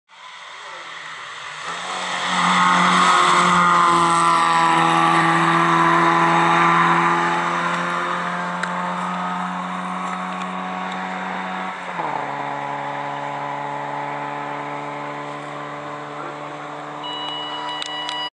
MG Midget almost "bites" the tar while cornering Simola
Watch near tar touching cornering by this 1964 MG Midget driven by Peter Hollis at the Simola hillclimb in Knysna.